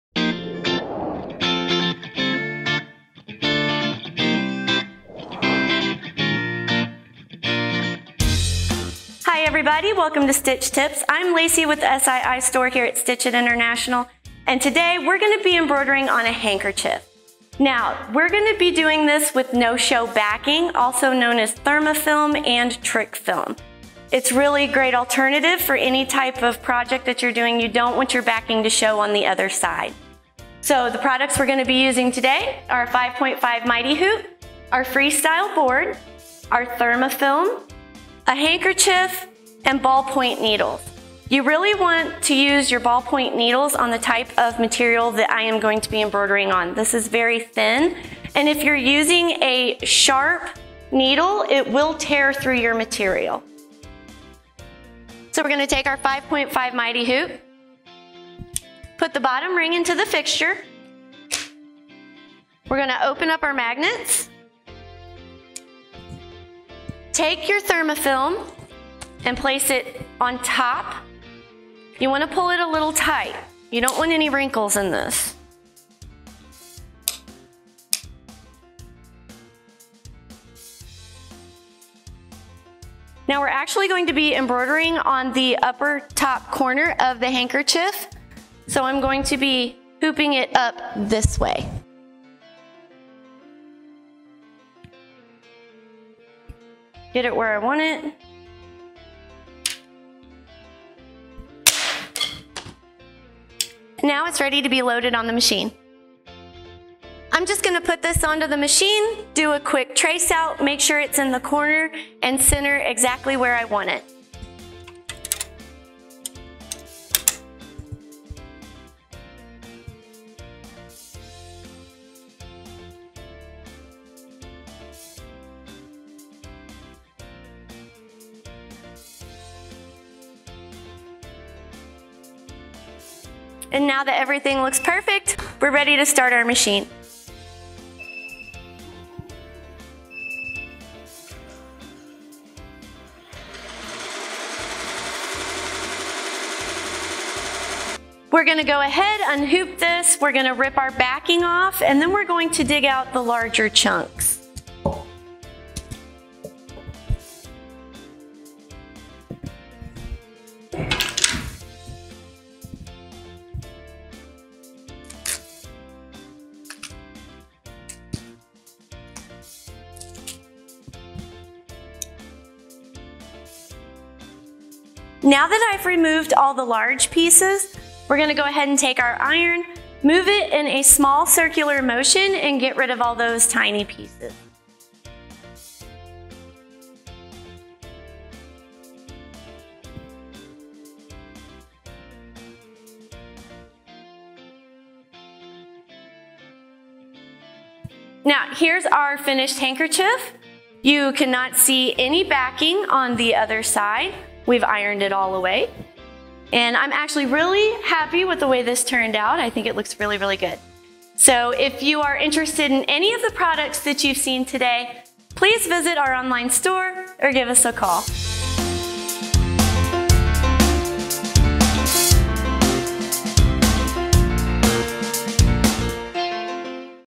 Stitch Tip: Embroidering on a Handkerchief (with No-Show Backing)
A monogrammed handkerchief is a great statement piece that’s delicate and can be a beautiful keepsake. The process of making a handkerchief requires just as much care as you put in to preserving them.

Use the right embroidery accessories and add on these extra tips that Lacey walks you through for a crisp finished design with no tears or damage to the handkerchief.